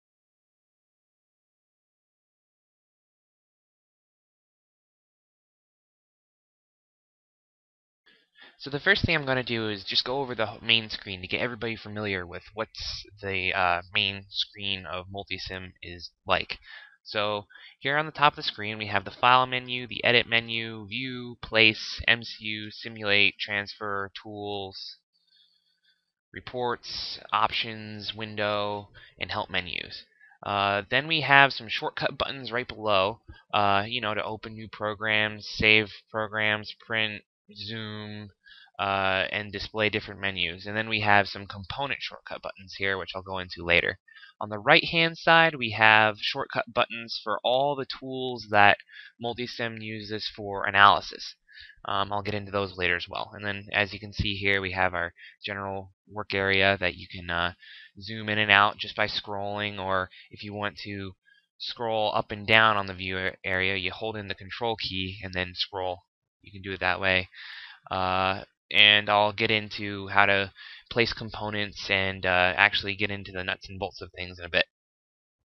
Multisim Main Screen
This shows you what all the icons are on Multisim 11 with a few helpful hints thrown into it.